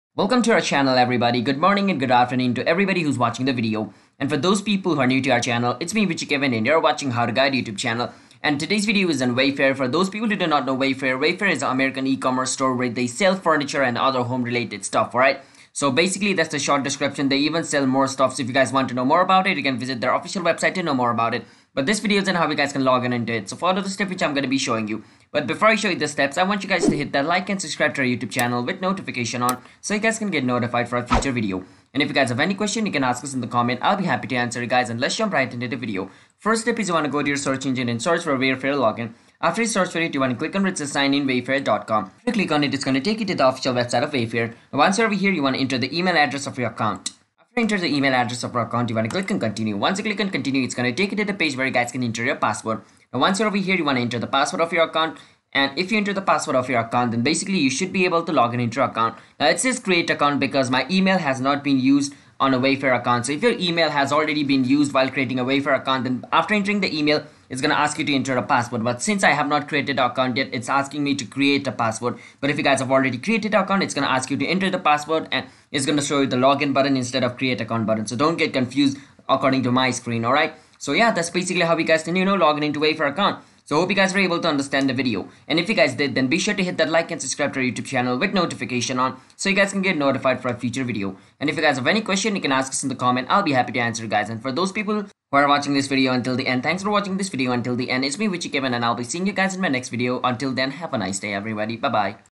How to Login To WayFair Account | WayFair Tutorial 2021
In this video, we'll show you how to log in and navigate your way through Wayfair's website so that you can find anything from a sofa to an ottoman.
In this tutorial, we're going to talk about how you can easily sign in to Wayfair account so that you can start shopping online. It's never been easier than it is now with our easy steps.
2. Click on the login button in the top right corner of the page

0:00 - Intro
0:35 - How to Login To WayFair Account
1:23 - Outro